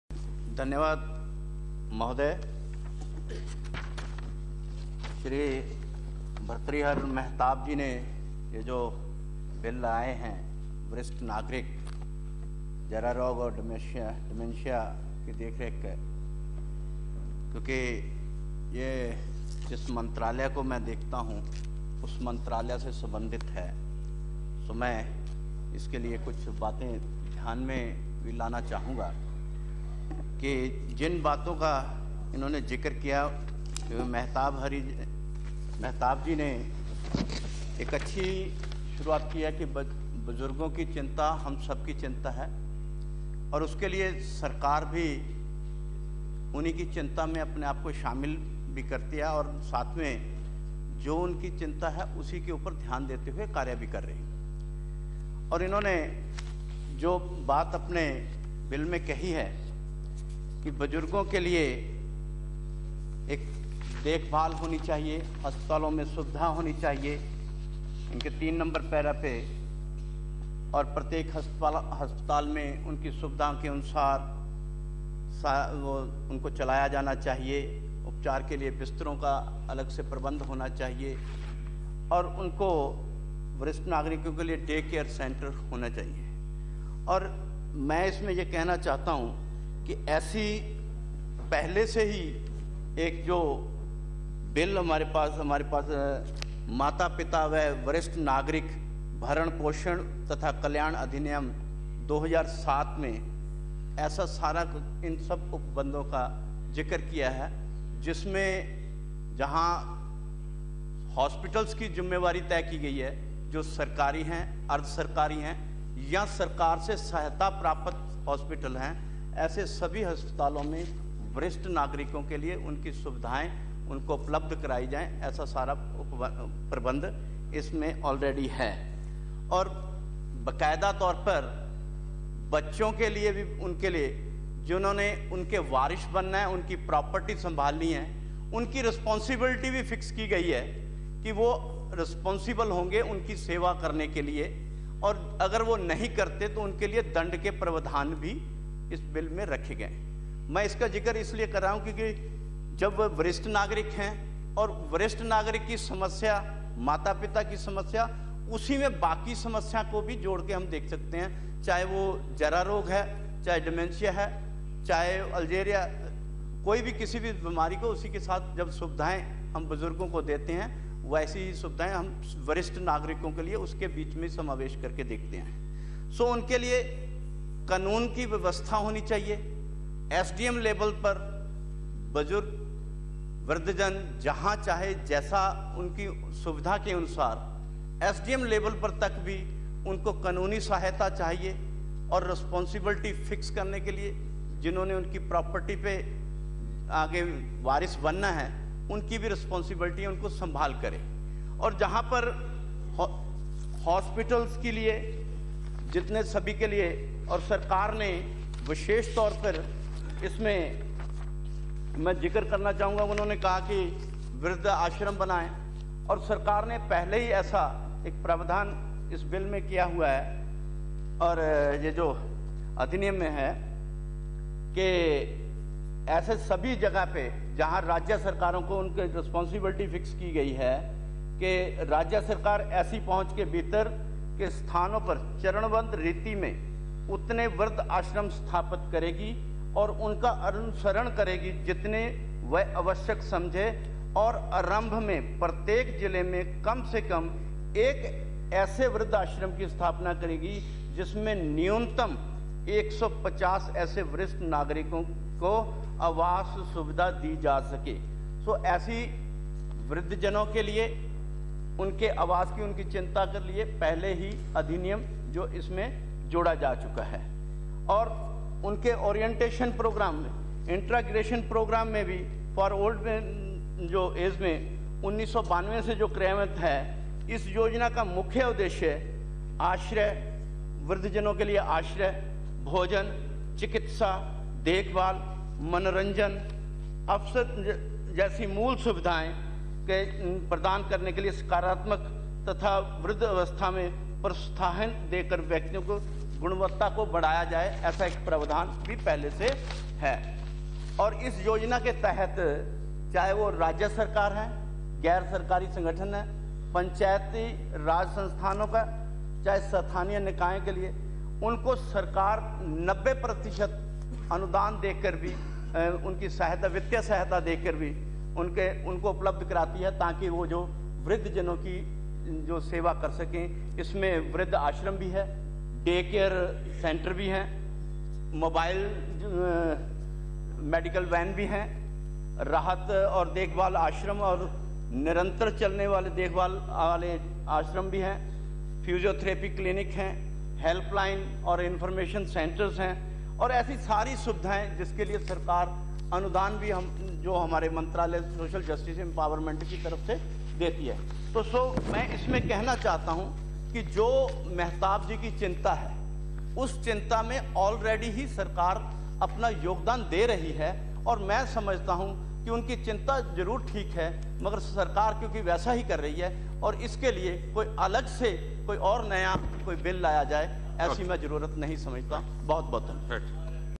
Discussion on Senior citizen (Provision of Geriatric & Dementia care) Bill 2014: Shri Vijay Sampla
13 March 2015, Private member's legislative business: Discussion on Senior citizen (Provision of geriatric and dementia care) Bill , 2014: Shri Vijay Sampla